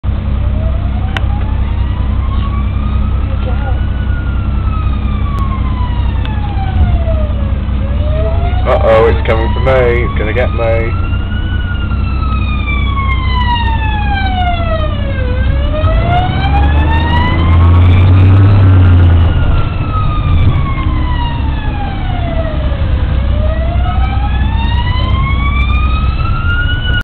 New Cop Car
Is This Devon And Cornwall New Weapon A Big Scary Cop Car?